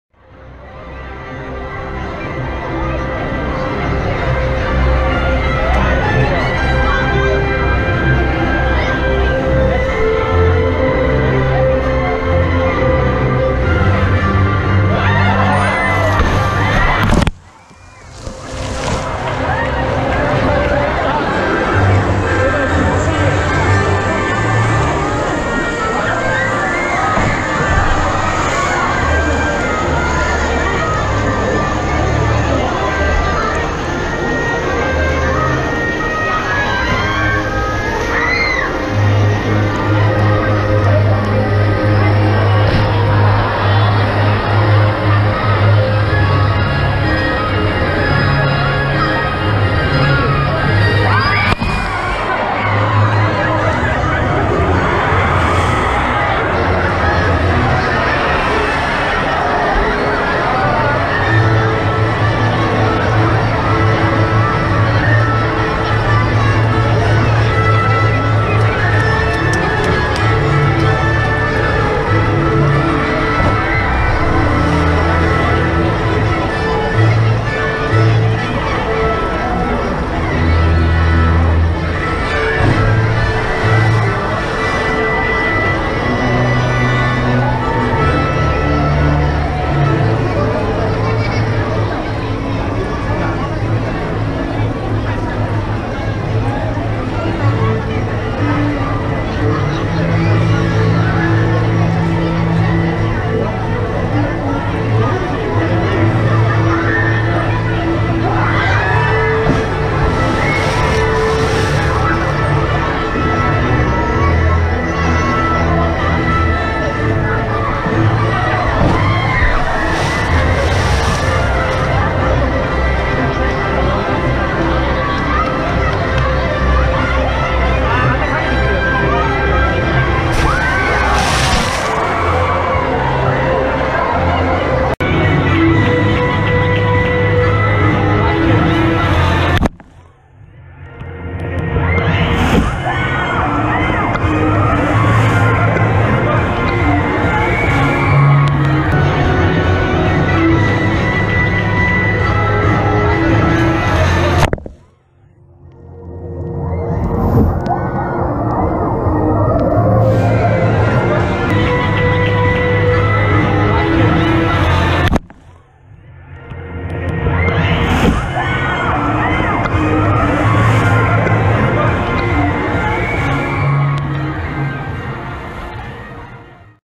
鴨川シーワールド２０２０ シャチパフォーマンス７５２ クッそ暑いので涼みに行きたいんだが
２０２０年８月１４日、１０時３０分からのシャチパフォーマンスショー
右サイド現在の最前列デンジャラスゾーンからローアングルバージョン。

カモシーの夏の新様式は、一人１日１回限りの整理券配布システムで、爆裂サマースプラッシュも無し！ブッカケも少なめ！ショーも短め！

撮影機材　gopro HERO5 session   Manfrotto  PIXI  MTPIXI-W